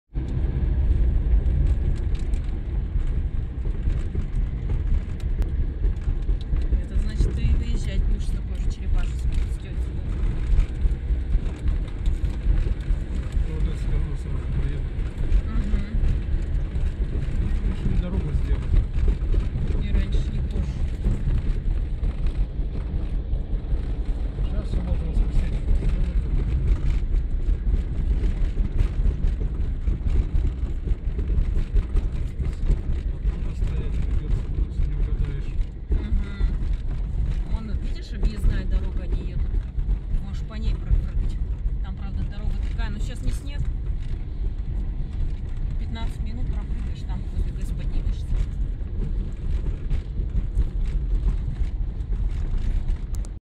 2018-09-29 Трасса Вилюй. Братск. Ремонт возле ГЭС +12° C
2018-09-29 The Vilyui highway. Bratsk. Repair near hydroelectric power station